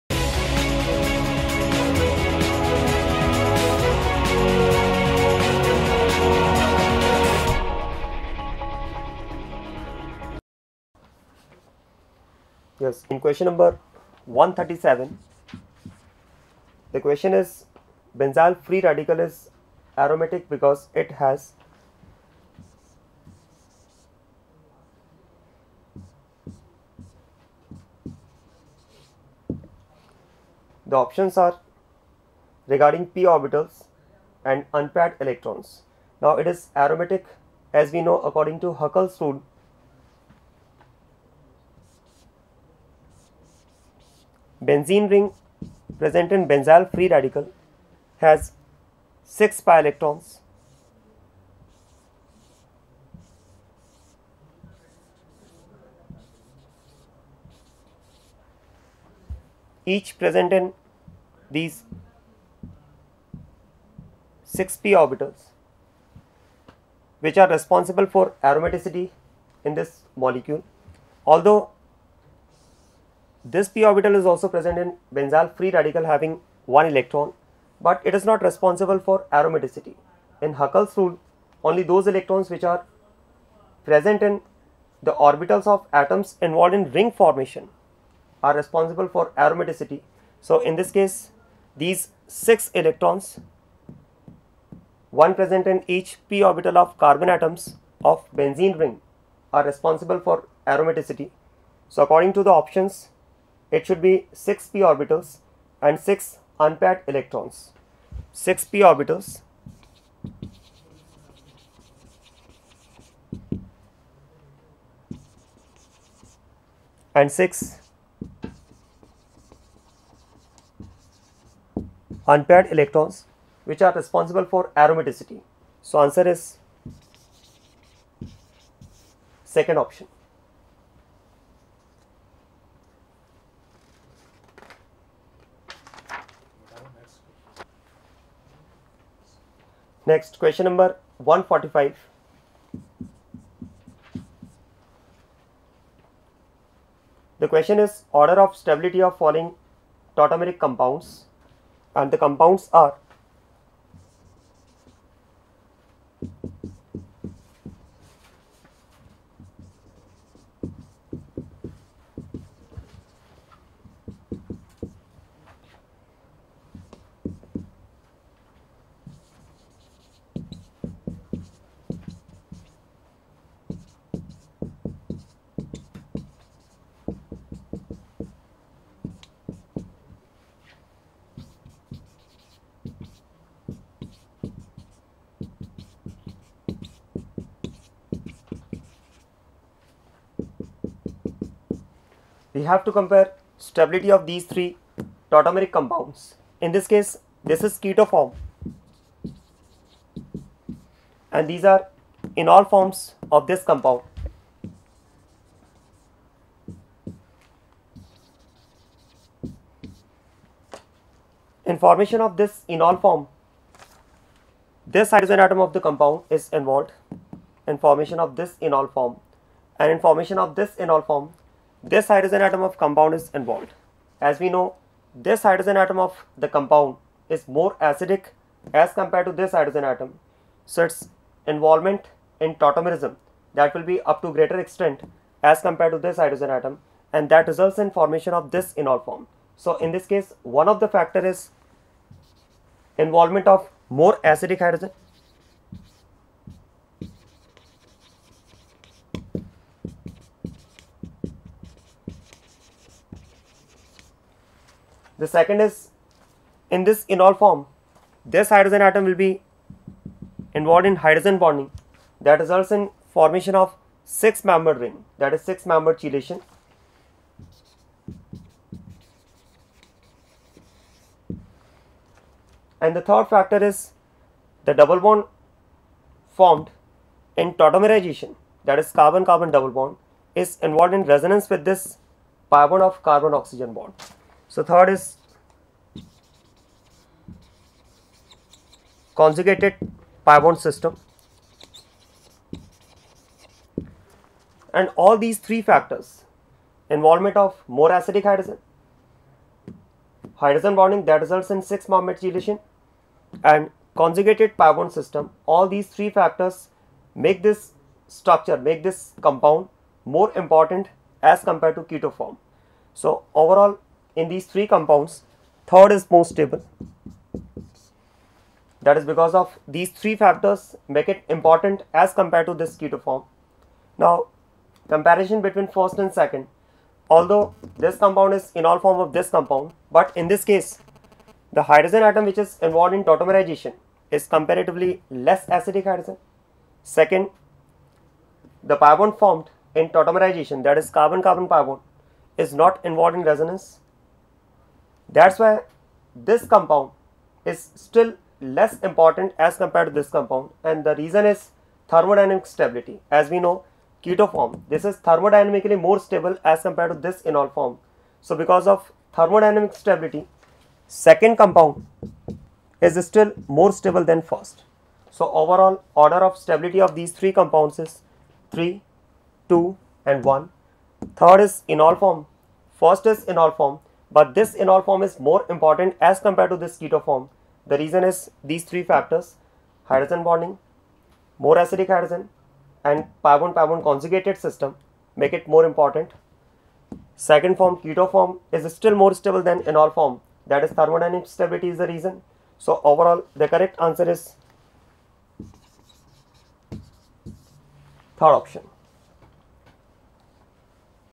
Chemistry Ques: 137,145 (NEET-UG 2013)
Video Solutions for NEET-UG 2013, by ALLEN Career Institute Kota, held on 5th May 2013.
Subject : Chemistry
Question nos. 137,145
Solutions done by Jitendra Chandwani Sir (Faculty -ALLEN Career Institute)